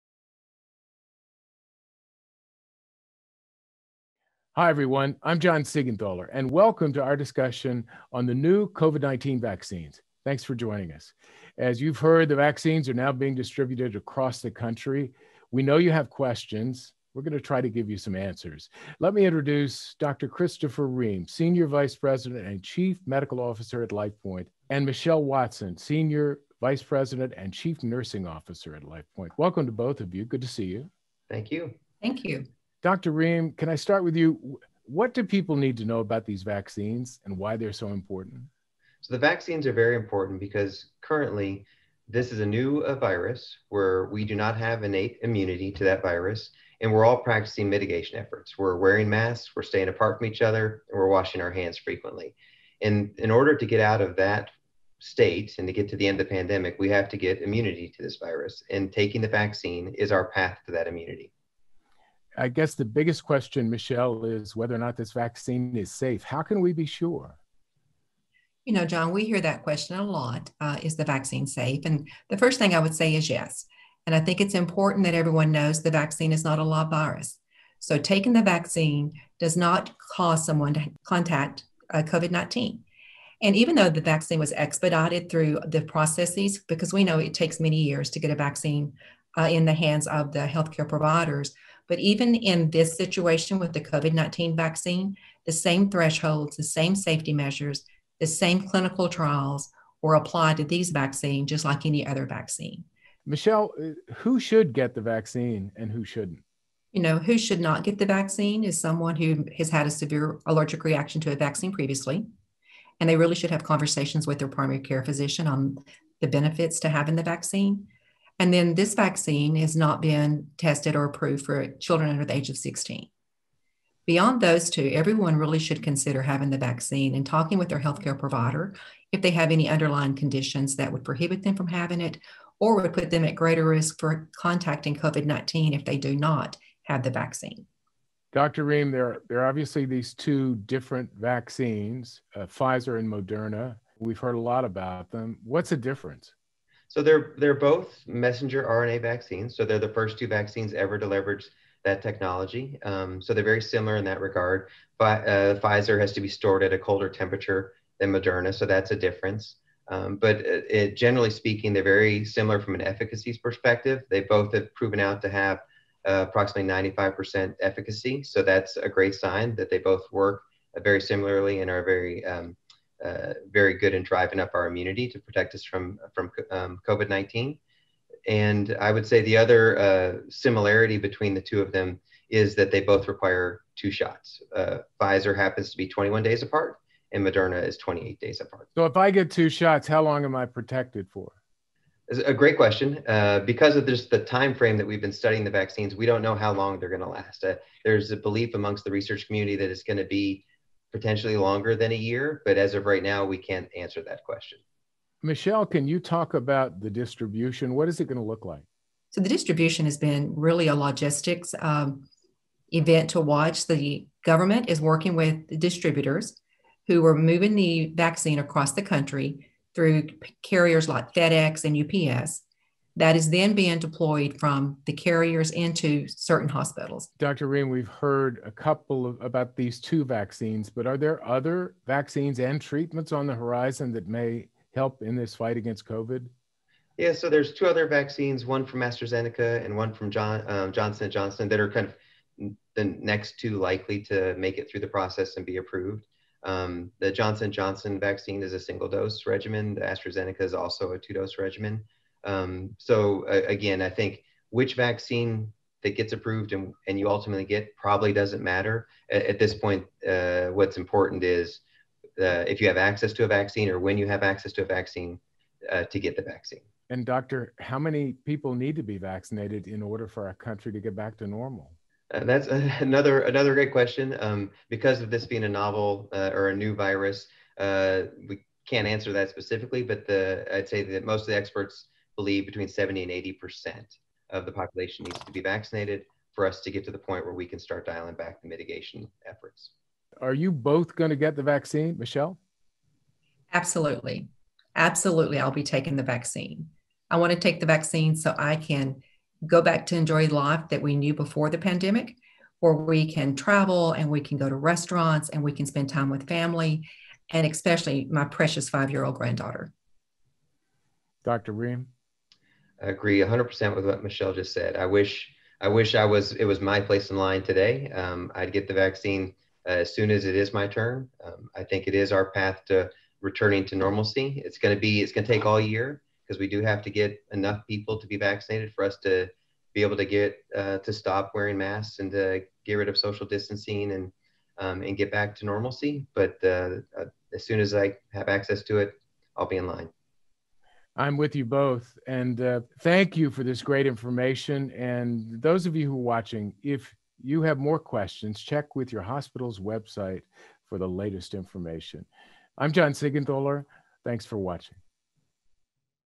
COVID Vaccine Zoom Q&A
Have questions about the COVID-19 vaccines? Listen in as LifePoint Health Chief Medical Officer Dr. Christopher Rehm and Chief Nursing Officer Michelle Watson, RN, MSN answer some of the most commonly asked questions about COVID-19 vaccination and discuss its safety and effectiveness.